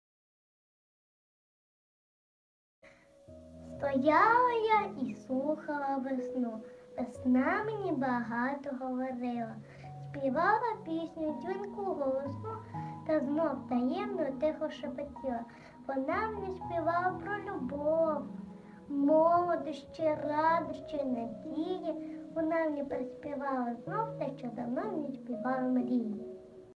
Остапенко Таміла Юріївна "Стояла я і слухала весну" | Конкурс «Змагаймось за нове життя!» 2024 рік
Номінація

Остапенко Таміла Юріївна
"Стояла я і слухала весну"
Вікова категорія: дошкільнята

Підготувала - Шаблій Надія Володимирівна, Тернопільський заклад дошкільної освіти (ясла-садок)№36, вихователь, спеціаліст вищої категорії

Заклад - Тернопільський заклад дошкільної освіти (ясла-садок) №36 Тернопільської міської ради

* Родини Бурбак.